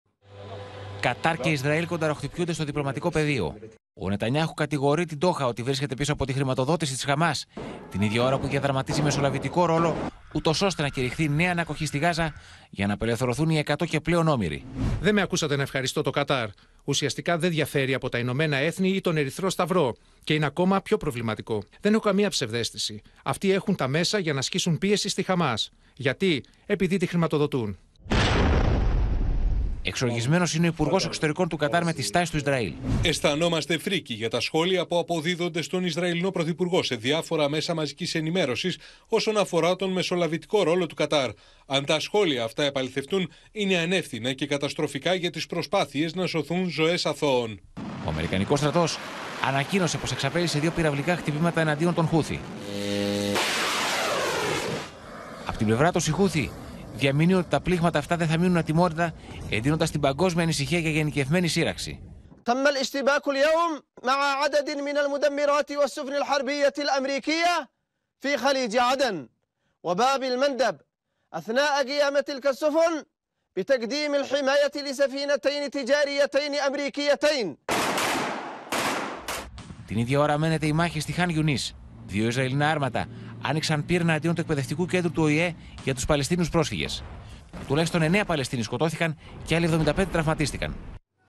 Ο Νετανιάχου κατηγορεί το Κατάρ για χρηματοδότηση της Χαμάς | OPEN TV
Στον αέρα βρίσκεται πλέον η προσπάθεια επίτευξης συμφωνίας με τη Χαμάς, μετά τις ισραηλινιές καταγγελίες πως το Κατάρ βρίσκεται πίσω από τη χρηματοδότησή της παλαιστιανικής οργάνωσης. Πλέον, οι ελπίδες για μια μακράς διάρκειας εκεχειρία με ανταλλαγή ομήρων - κρατουμένων μεταξύ των δύο πλευρών, μειώνονται δραματικά.